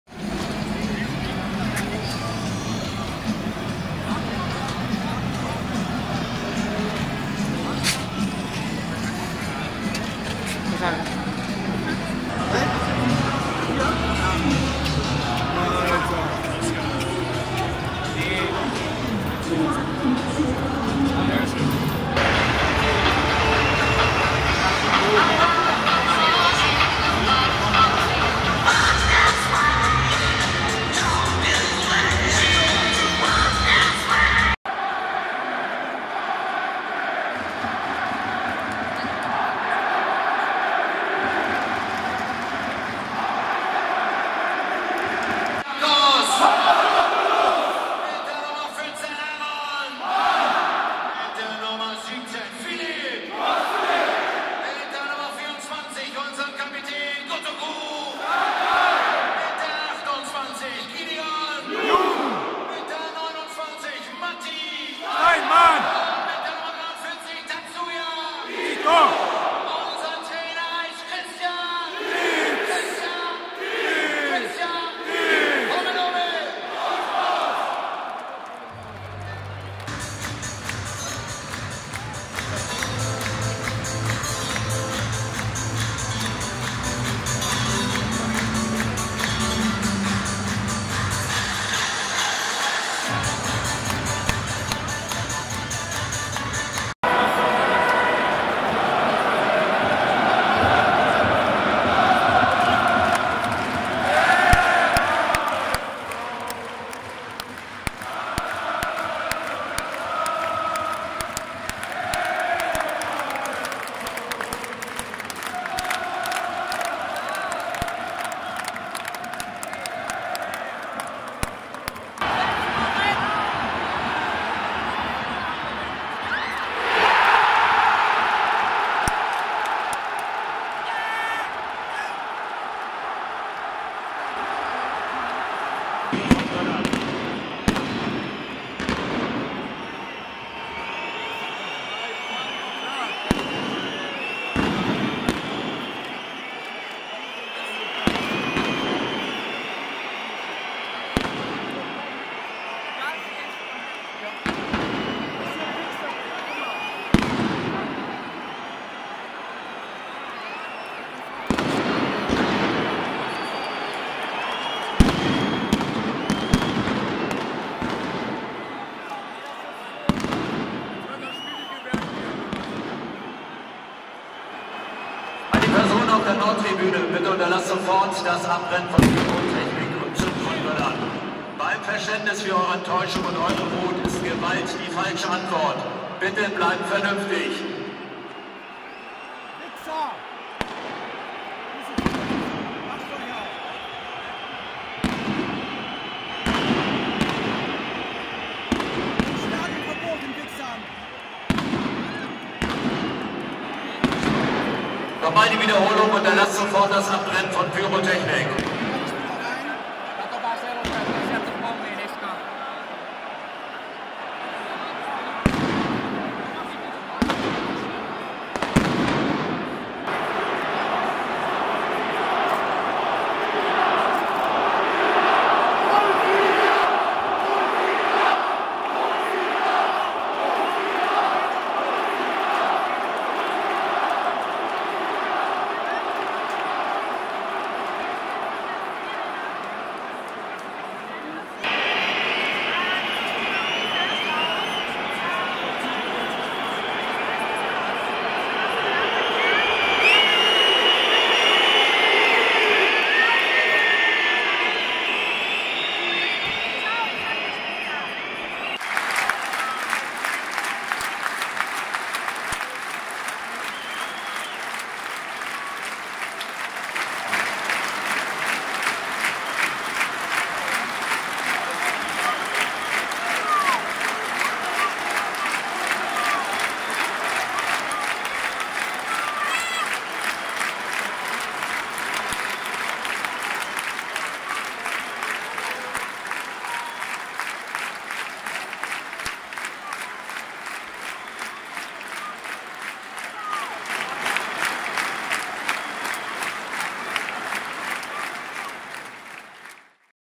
HSV-Mönchengladbach 12.5.2018: Relegation and angry ultras
Finally put my clips together from last summer's match in Hamburg. HSV relegated from Bundesliga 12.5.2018.